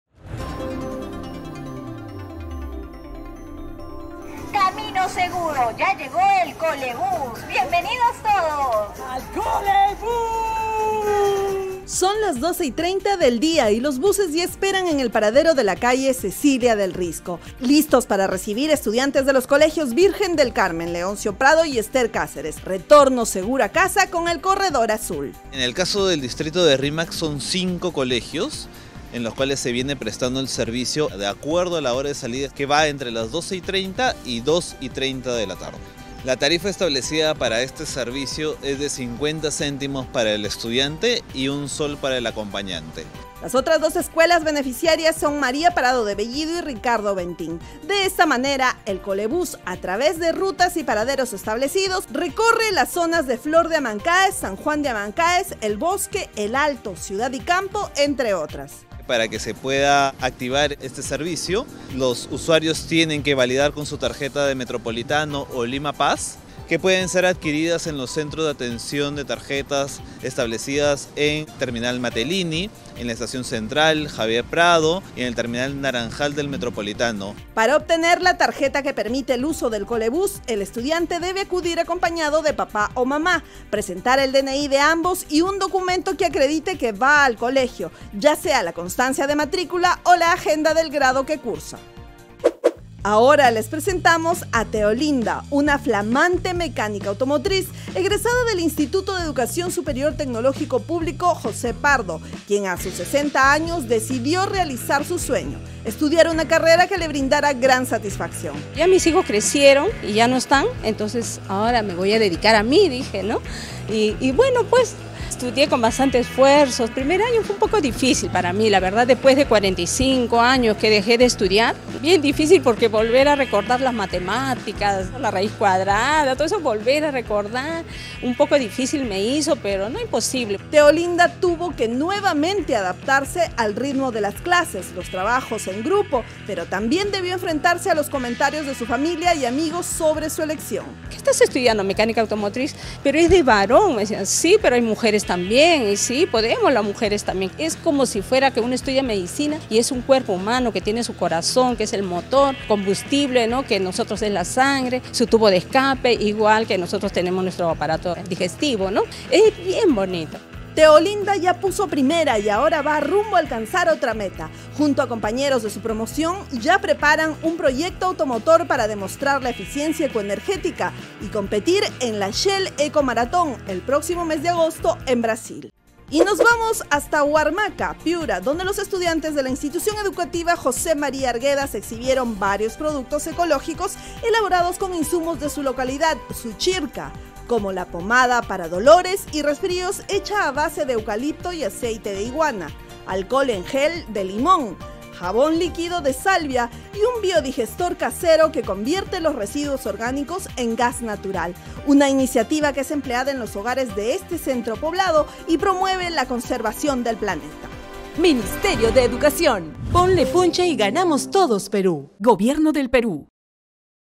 El Colebus
MineduInforma | 🚌 Llega el Colebus, la solución perfecta para estudiantes como tú. Con tu tarjeta del Metropolitano o Lima Pass, disfruta de un viaje cómodo, económico y seguro para ir al colegio y regresar a casa. 📚🏫